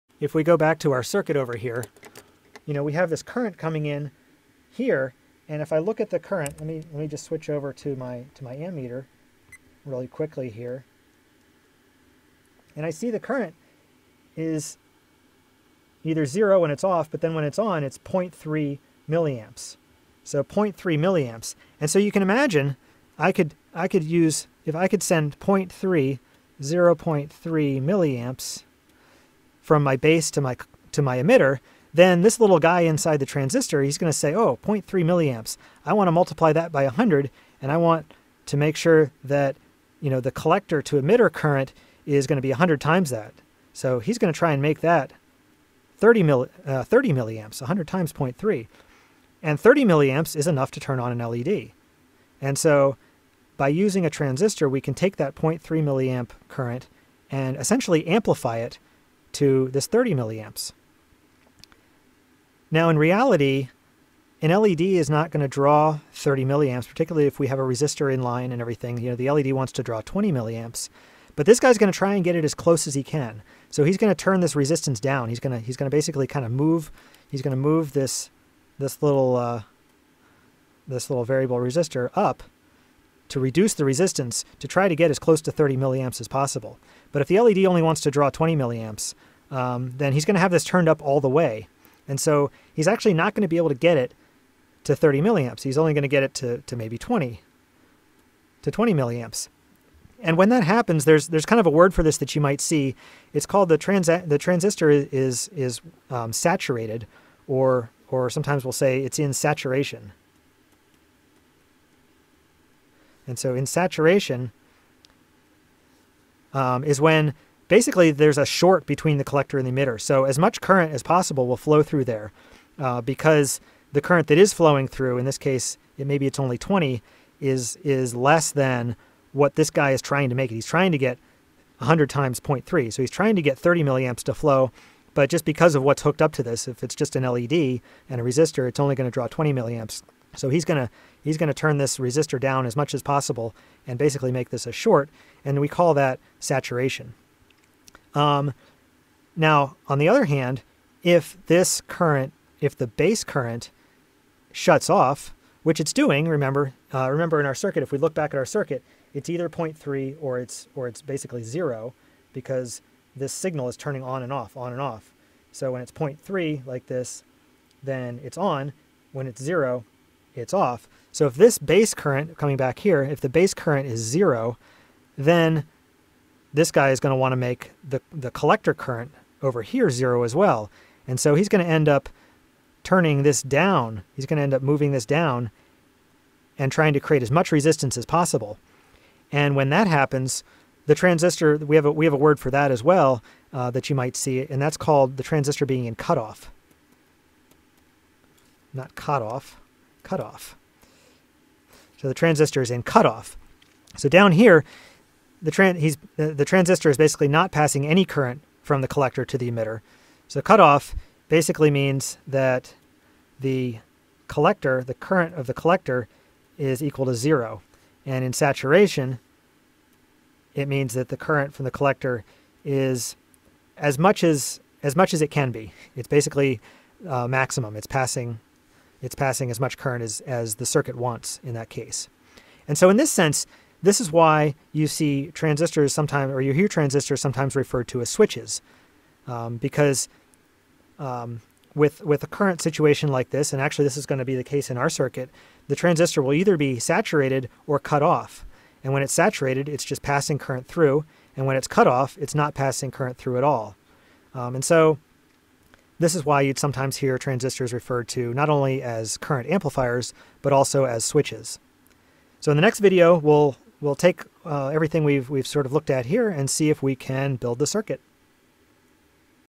The transistor as a current controlled switch | Digital electronics (7 of 10)
Often times transistors act more like switches. Here we see why. Thinking of transistors in this way might make digital circuits easier to understand.